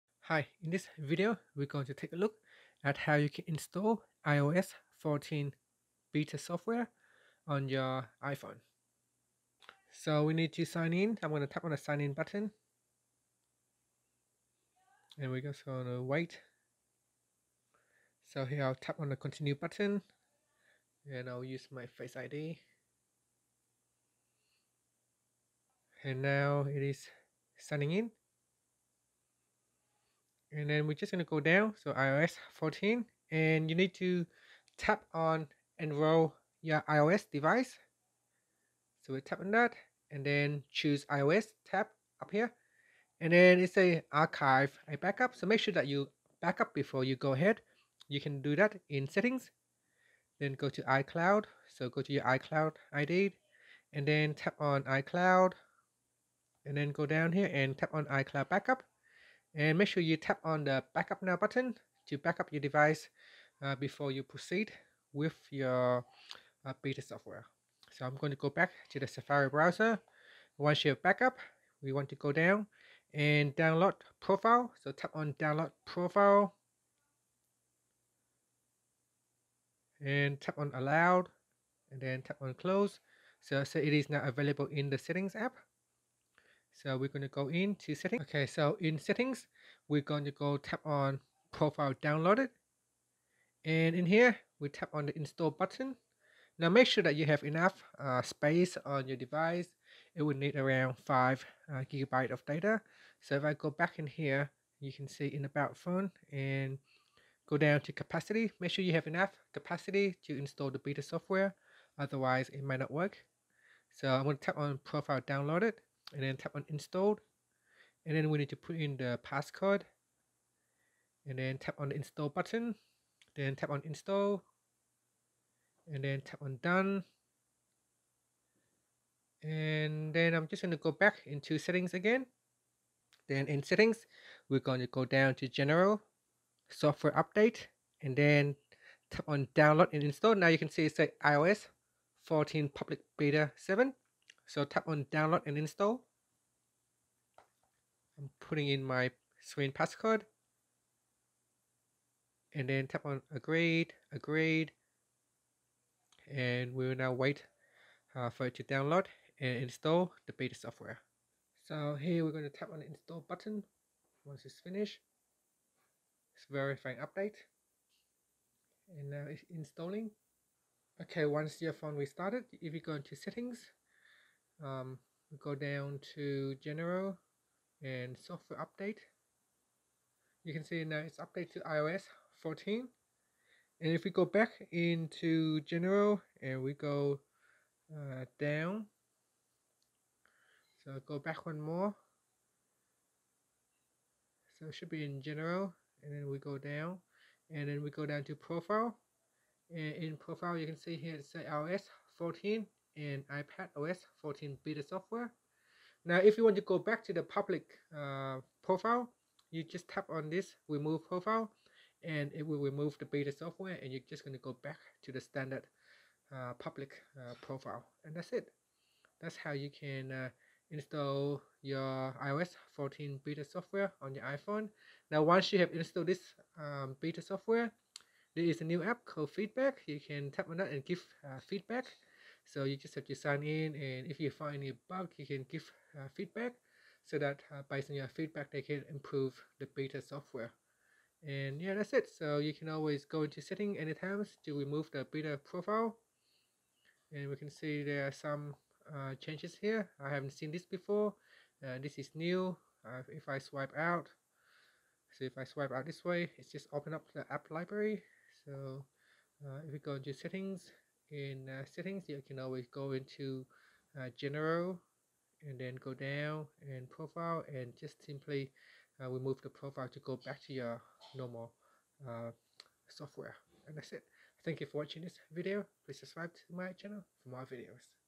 How to Install iOS 14 Beta Software Quick
Learn how you can quickly install iOS 14 Beta software so you can test it out and see all the new features before the actual public release of the final version.

iOS 14.